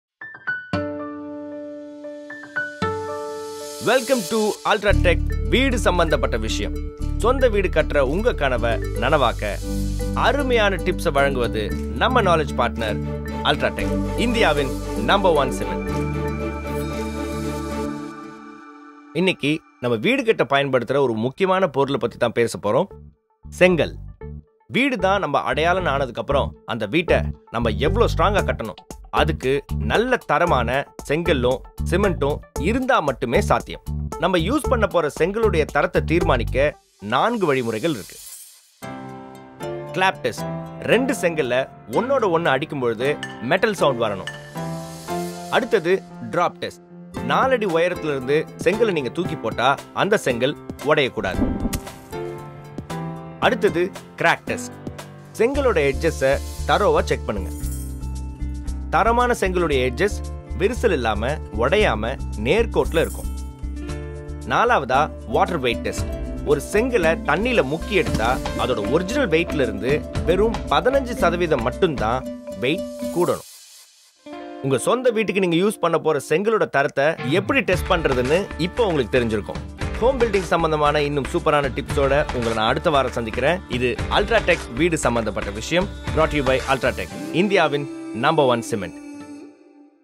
BaatGharKi on Star - Brick Testing (Tamil)
While building your dream home it is necessary to ensure the strength and durability of it. Let us understand how to test and choose the right brick with Ma ka pa. To know about home-building follow
for claim details.